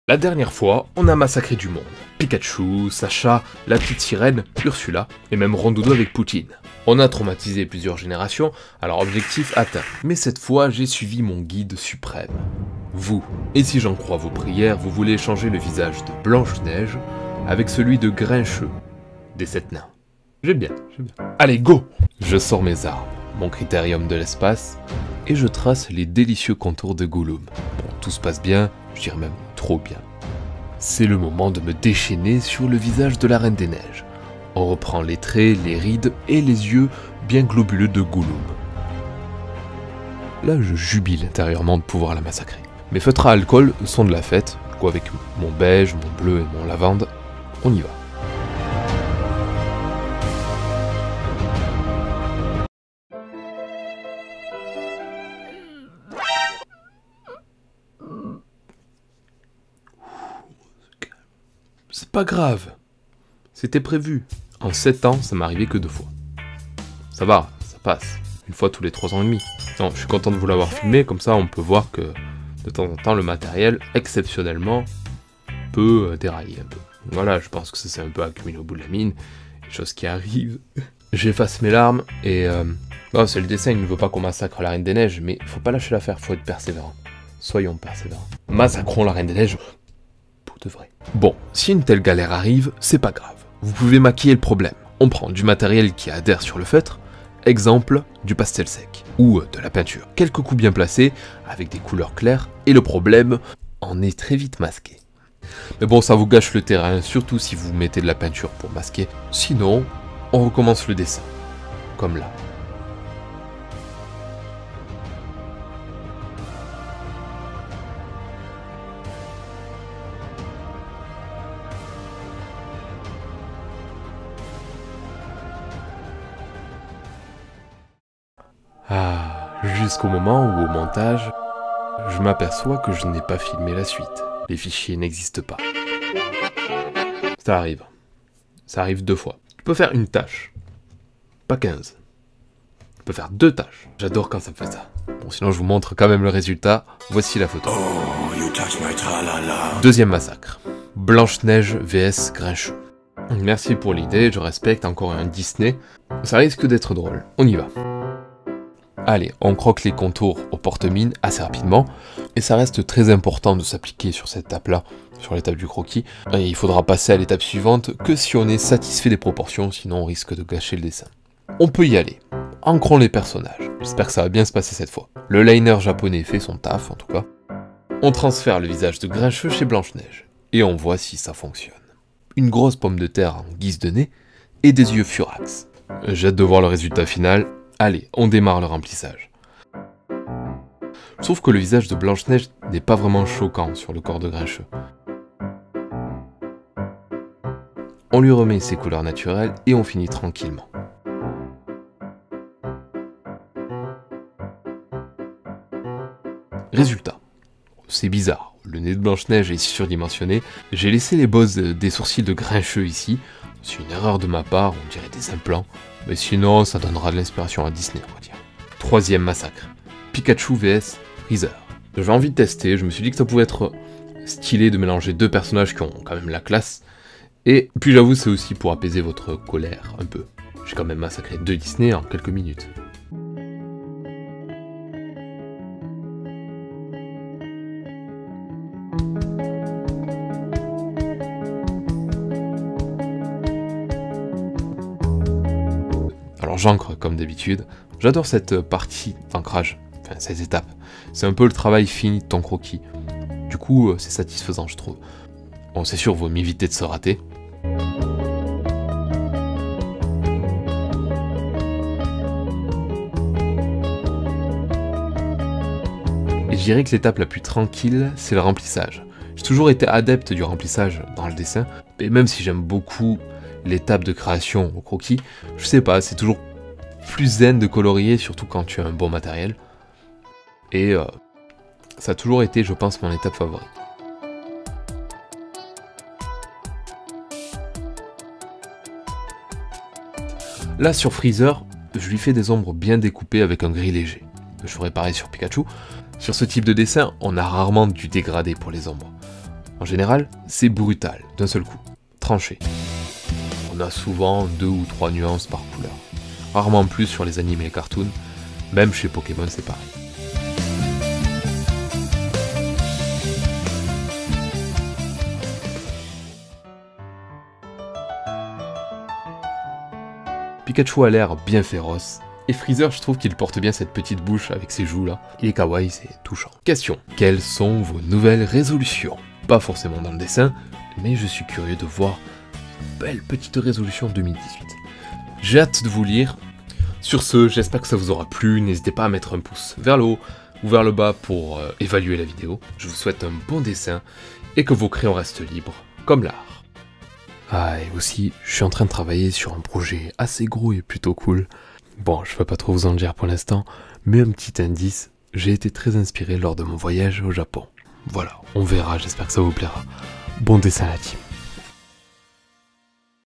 UN ACCIDENT DE DESSIN :'o (Fusion challenge) 2
Prenons deux personnages, inversions les visages et regardons
ce que ça donne ! O_O

Musiques cc:
Episode 2 Challenge fusion  : J'échange leur visage en dessin !
En espérant ce trio de fusions vous plaise !